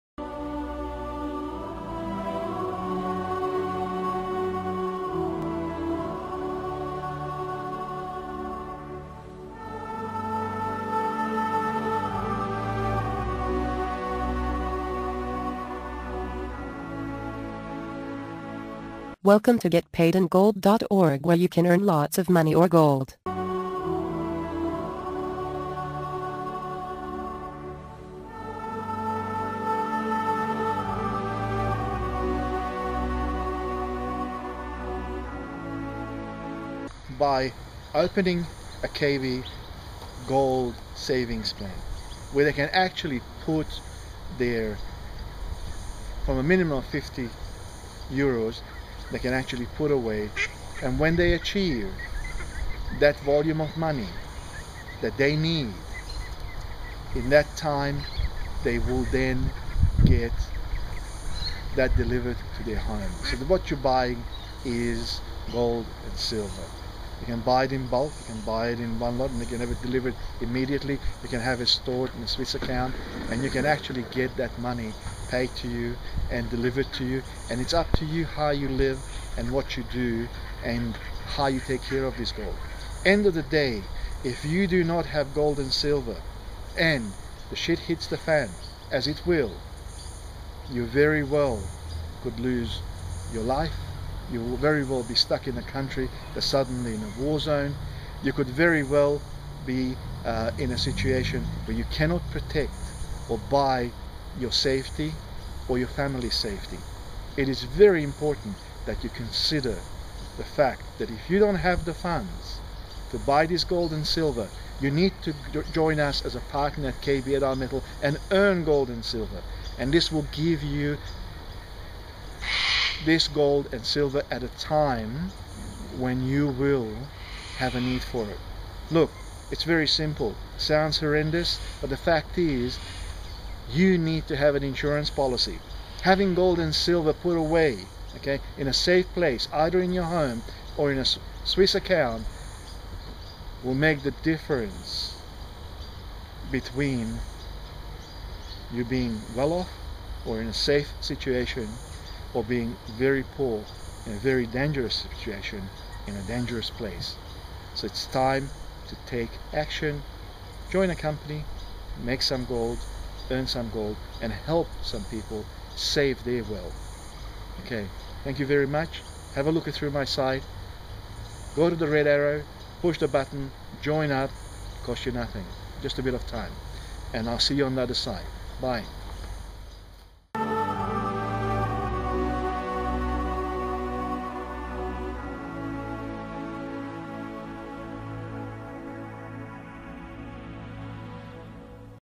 Simple Ways to Buy Gold
No Fees, No Monthly Costs, No Obligations!..Earn .. €.. or Gold...Fiend out How...
Being new in gold can be confusing and intimidating. Many people tend to over-complicate things about gold. Having a good sense of owning gold is an essential start in gold . Buy gold bullion is the best way to do it.

Here are some tips on how to buy gold:

6. Familiarize yourself with gold bullion.
7. Familiarize yourself with the current gold prices.
8. Make sure to get your order at the current prices over the phone.
9. Make payment.
10. Wait for your order.

Essentially, there are three categories of gold investors:

4. People who want to make a profit.
5. People who wants a hedge against financial down turn.
6. People who want to combine a hedge and a future profit.

Your objectives will determine what you include in your portfolio. Some thought and attention must be given to which of the three categories you belong. Once you have a handle on why you are buying gold, deciding what to buy becomes a relatively easy process that can be sorted out between you and your broker.

Gold has been the foundation of most national currencies for up to second half of the 20th century. Gold bullion is extremely stable hoard of value because of its desirability and tendency not to rust or corrode over time. Gold has proven to be a profitable in the past and it is still doing its job until now.

Gold is not only being form of savings in case there is a need of money. First you must understand what kind of investment you want to make in gold bullion so that you will distinguish the kind of gold you will purchase. Buy bullion now to make money or keep your funds safe.